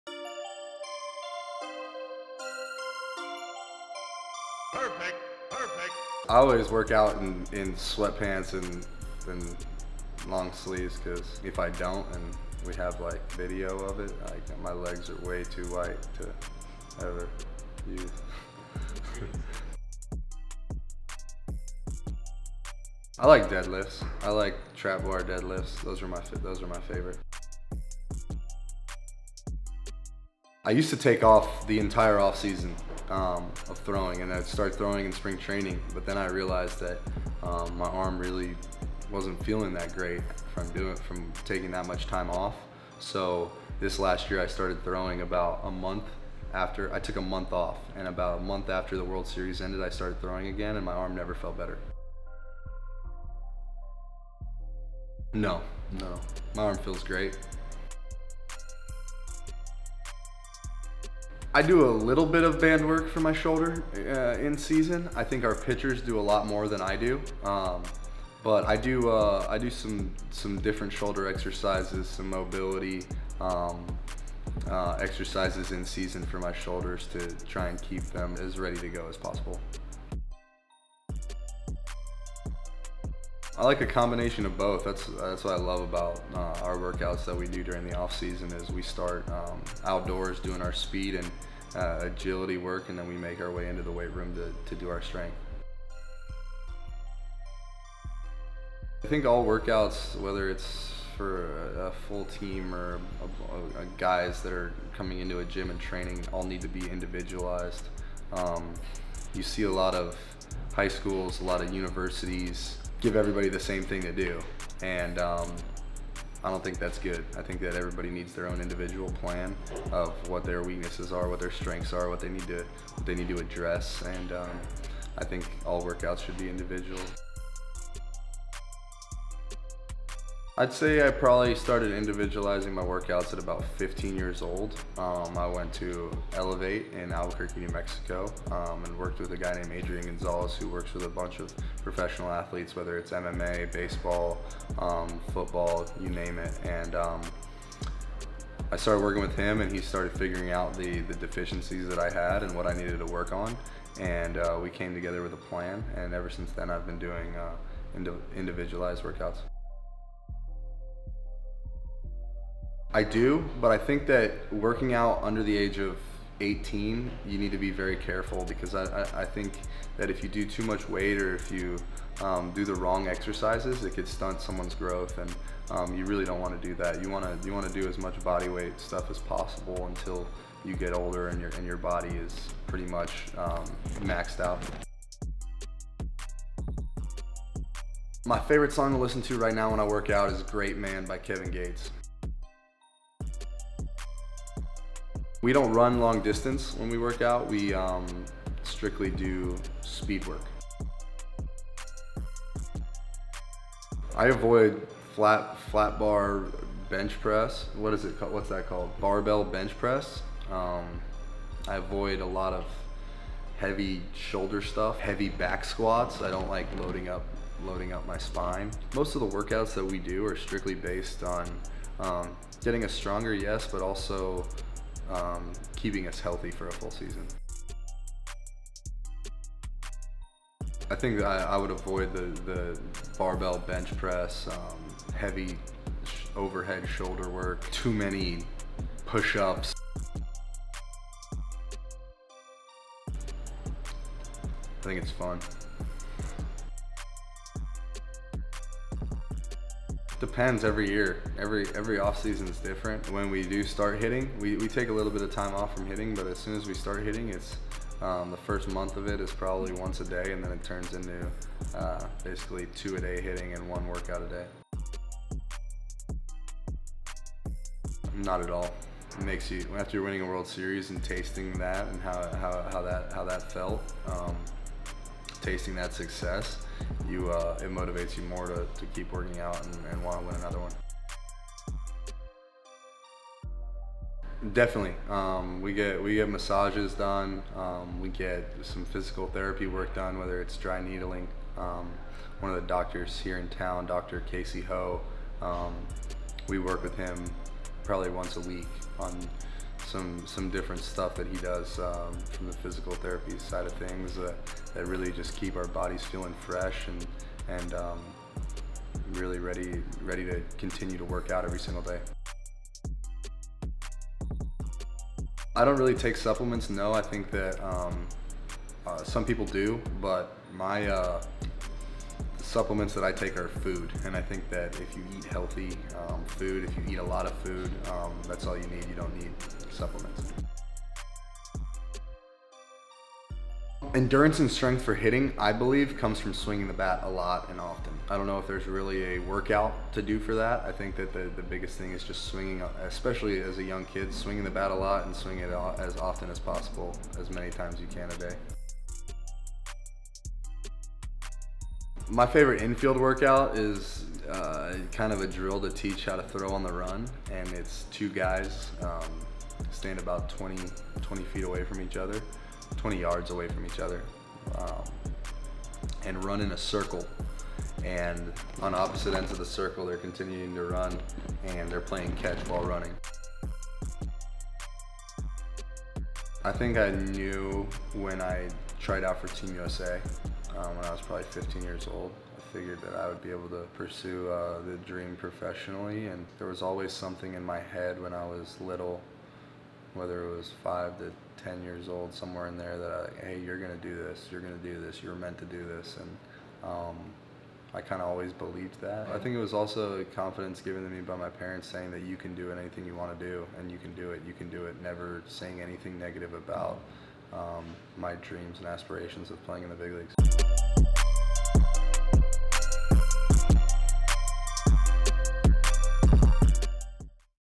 Does Alex Bregman Run? | Answering Workout Questions Part 2!
Fielded some questions from y'all on my Instagram story and answered them on camera! Ask me some questions on insta if you haven't already.